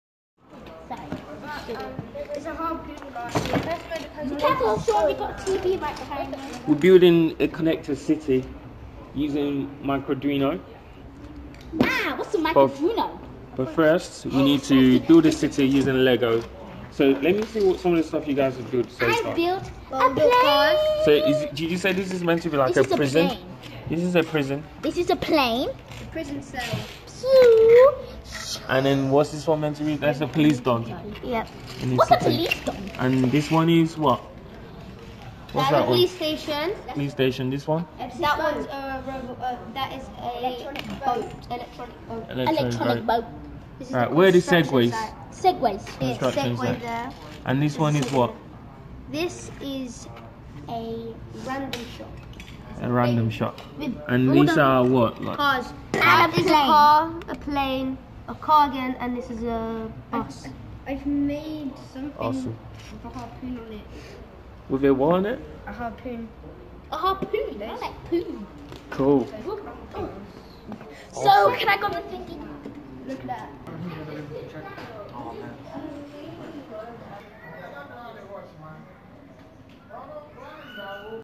Building a connected city using Microduino & lego
On our way to building a connected city using Microduino & lego electronics arduino. Today at Digilab